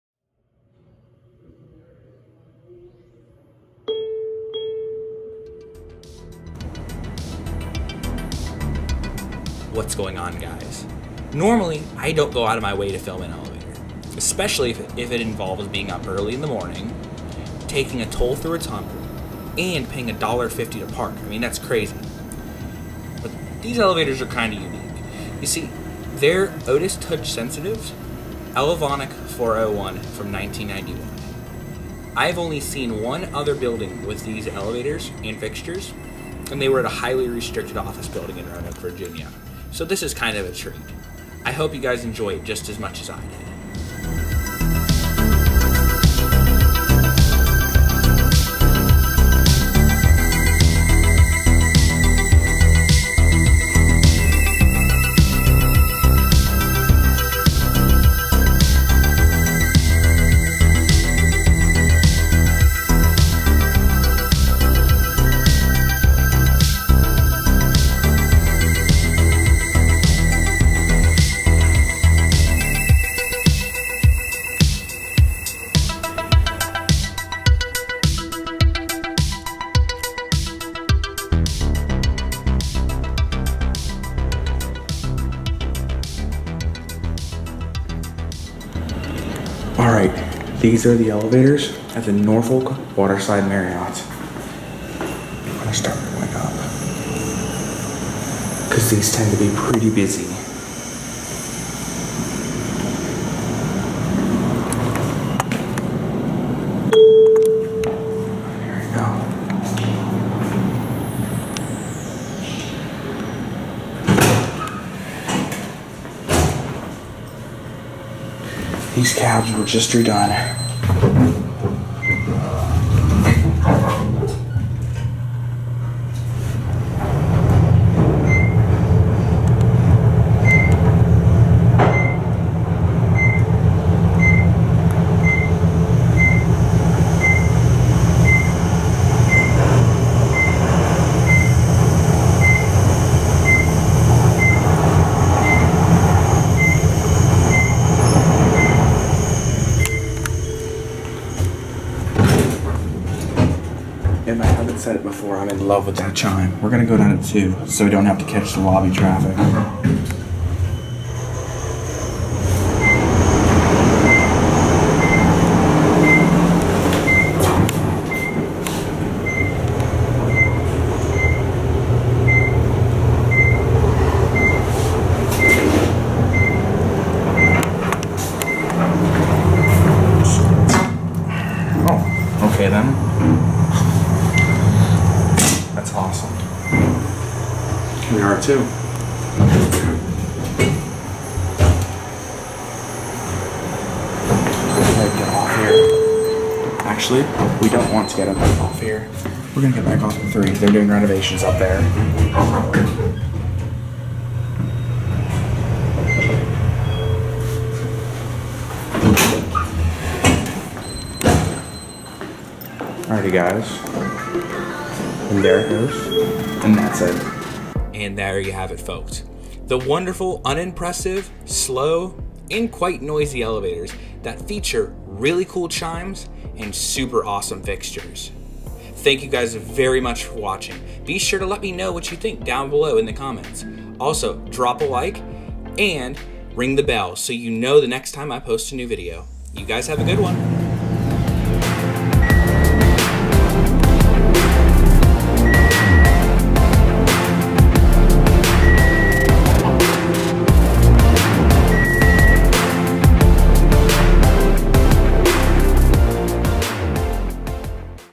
Rare Otis Touch Sensitive Elevators at Norfolk Waterside Marriott
I do apologize for the somewhat shotty camerawork, though I am happy to finally have a video of this elevator!